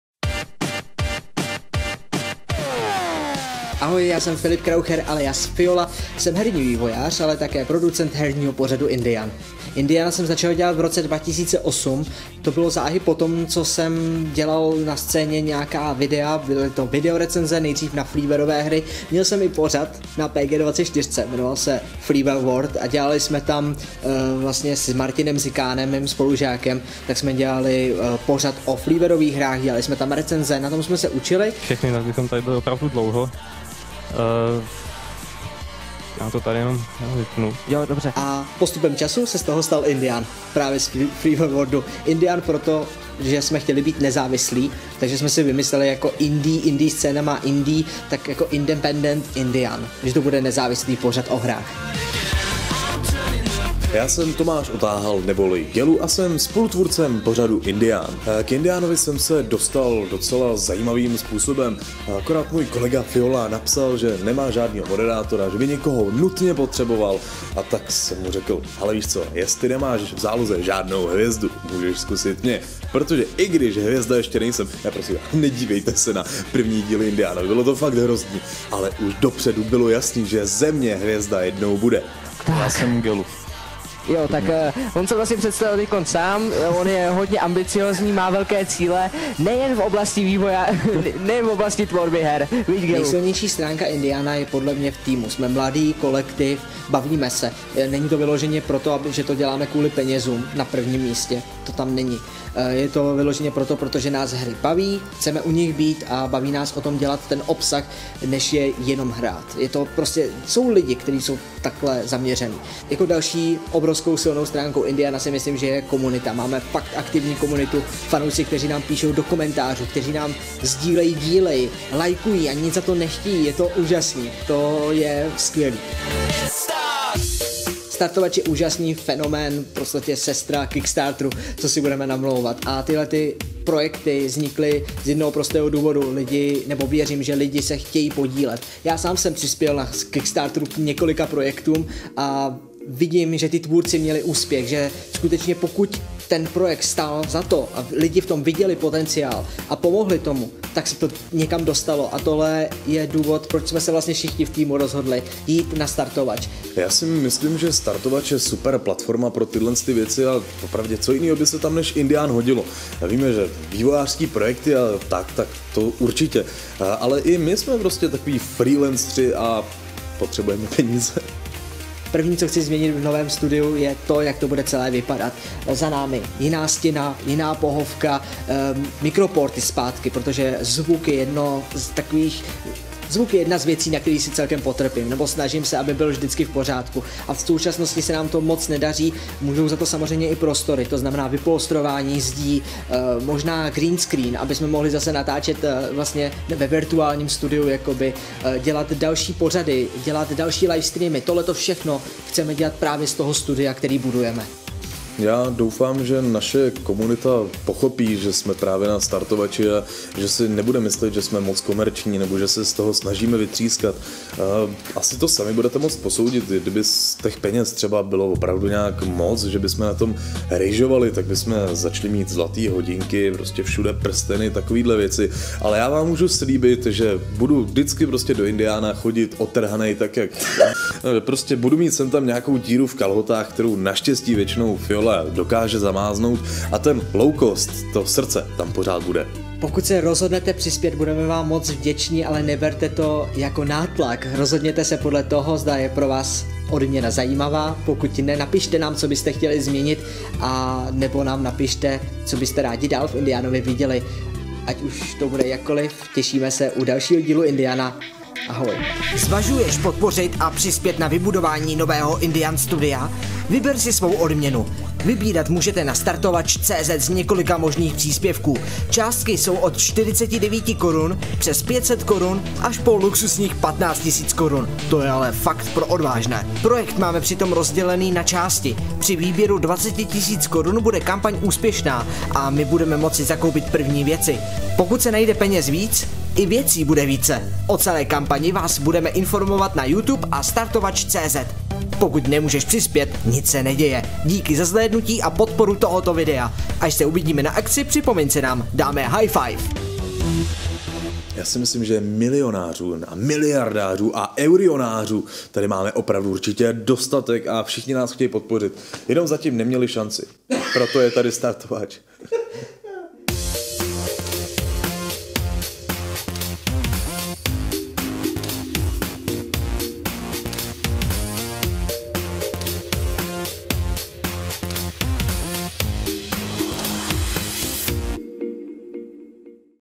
Herní pořad Indian
INDIAN je herní pořad, který od roku 2006 natáčel své díly ve studiu jiné společnosti. Nyní chce tým vybudovat studio vlastní. Pomozte i vy! Díky moc.